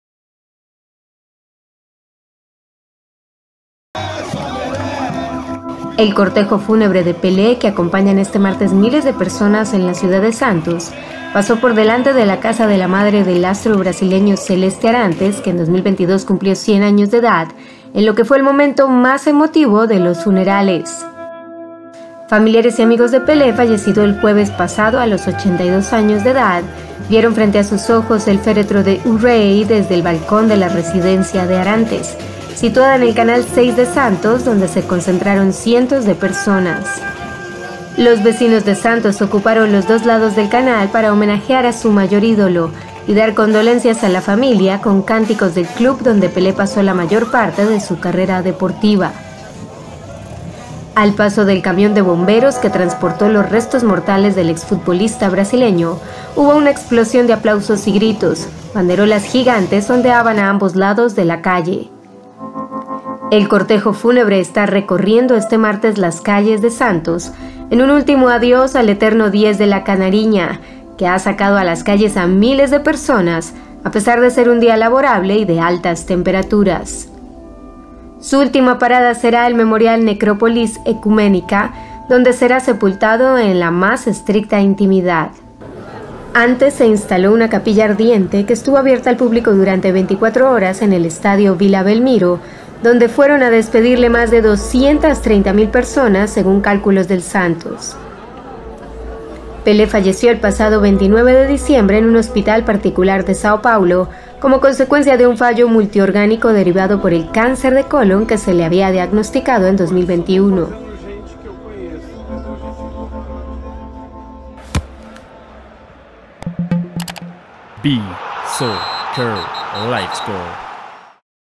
El cortejo fúnebre de Pelé conmueve a su paso por la casa de su madre
El cortejo fúnebre de Pelé, que acompañan este martes miles de personas en la ciudad de Santos, pasó por delante de la casa de la madre del astro brasileño, Celeste Arantes, que en 2022 cumplió 100 años, en el momento más emotivo de los funerales.